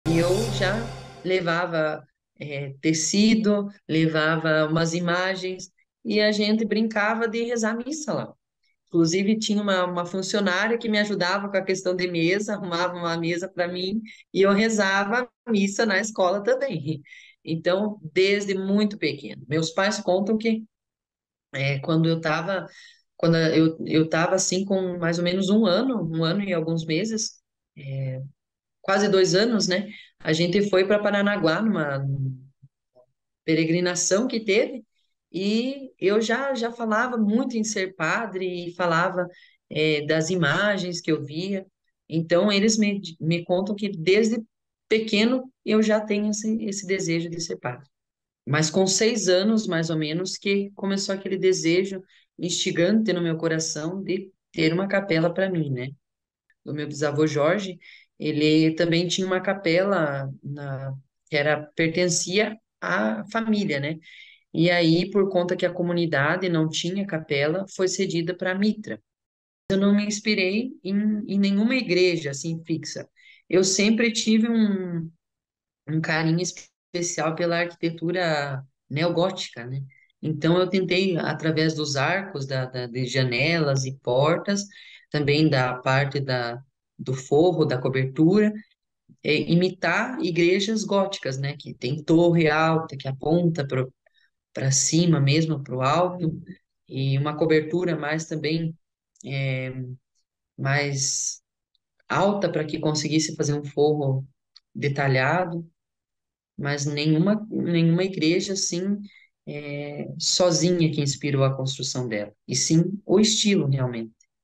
Menino constrói a própria igreja aos 13 anos no Paraná e sonha ser padre
Olá!